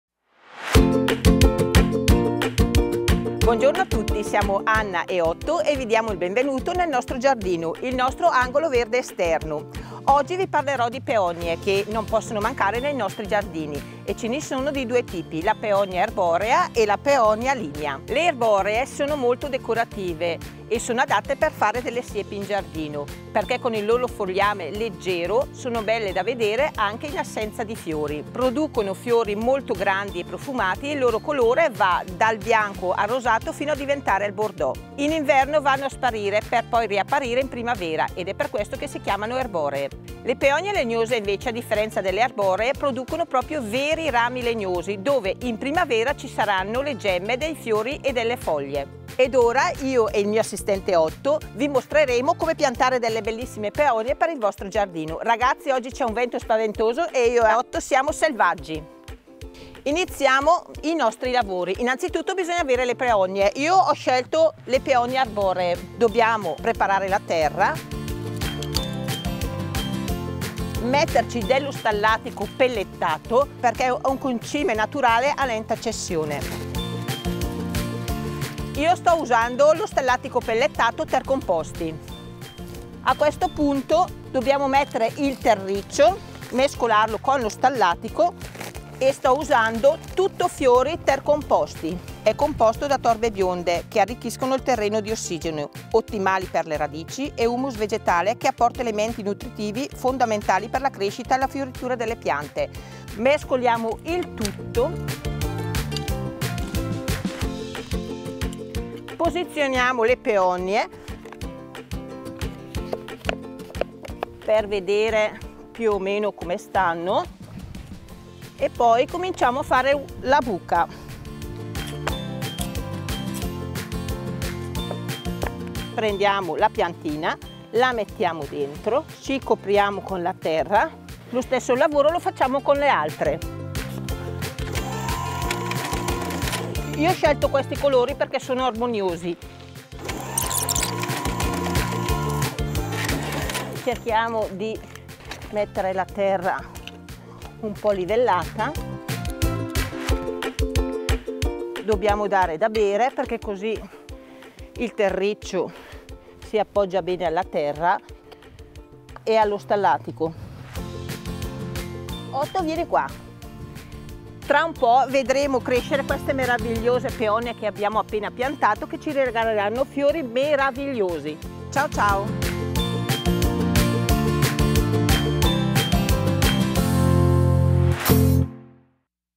L'angolo verde - Le Peonie
Benvenuti nel nostro Angolo Verde, all'aperto! Oggi Anna ci parla delle Peonie!